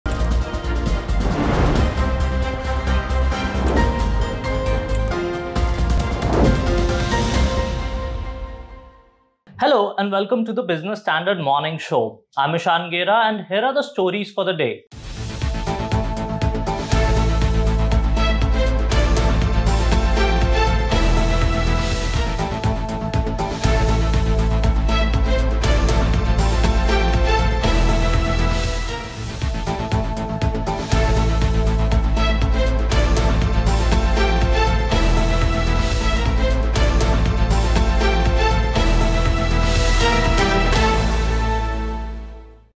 TMS Ep242: ‘One charger for all’, China 5G, Sensex rally
How will 'one charger for all' affect device makers? How were the Chinese frozen out of 5G? Can Sensex hit the 65,000 mark in the rally? What’s transfer and transmission of shares?